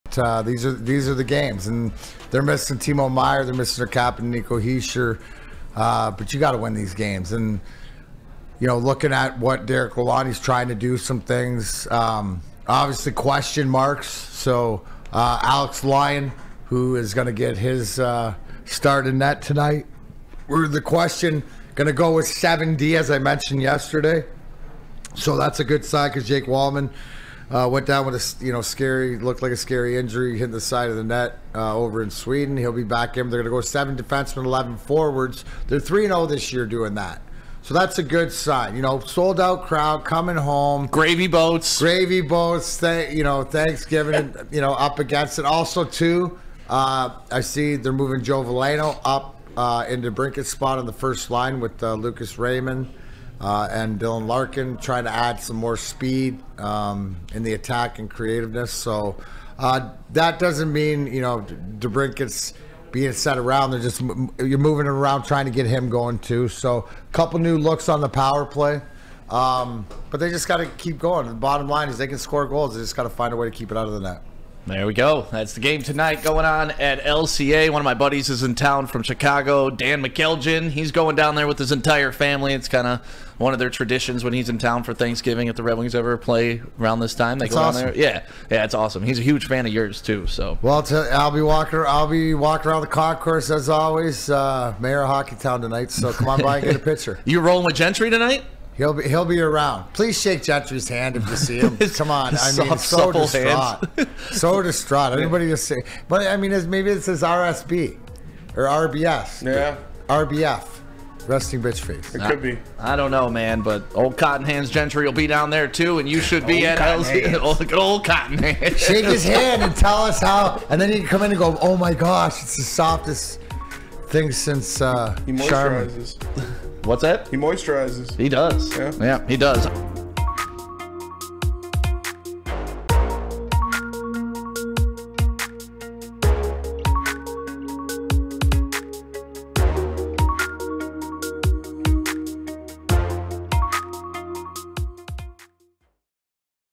The Detroit Red Wings are Shaking Things Up!
Darren McCarty and Sam "Stick" Day discuss how the Detroit Red Wings are testing out new looks.